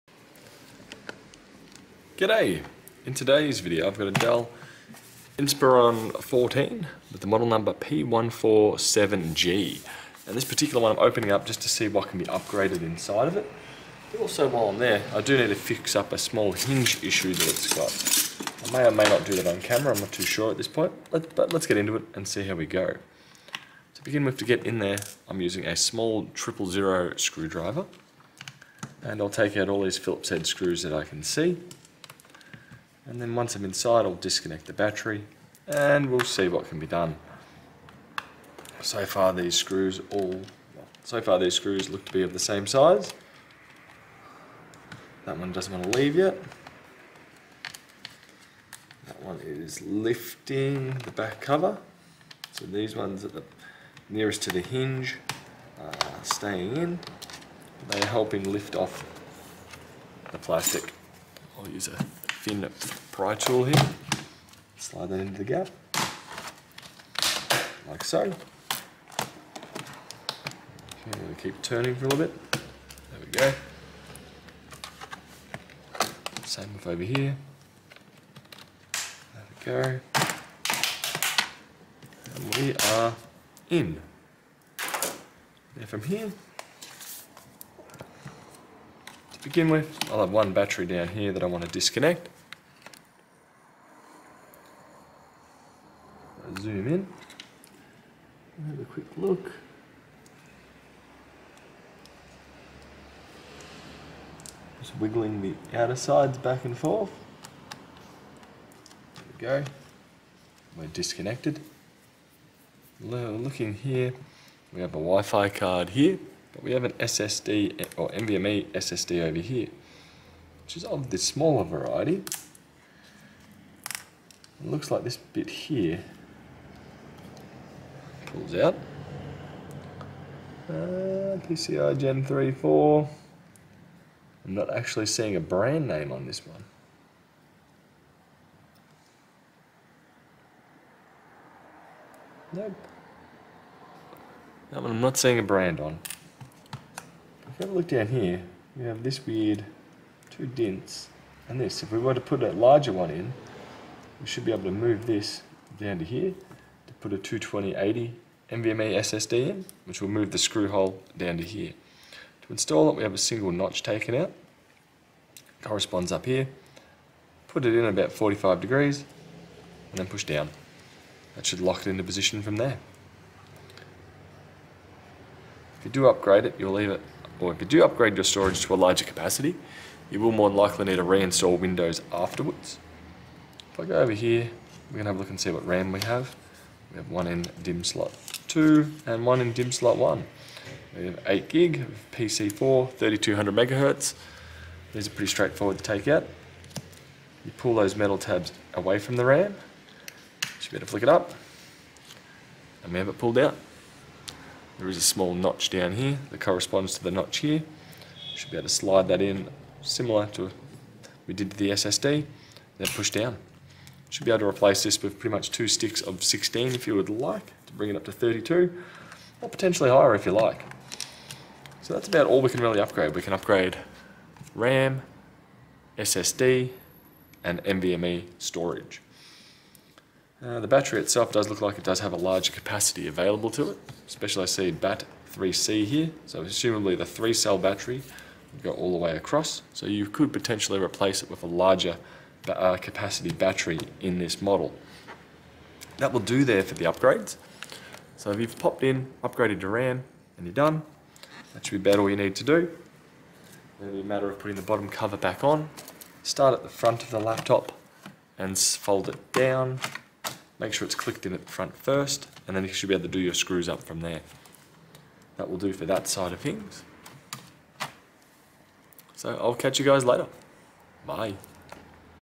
Dell Inspiron 14 P147G Ram and NVMe upgrade discussion
In this video im opening up a Dell Inspiron 14 P147G to fix the broken hinge mount but while im inside i will also cover what can be upgraded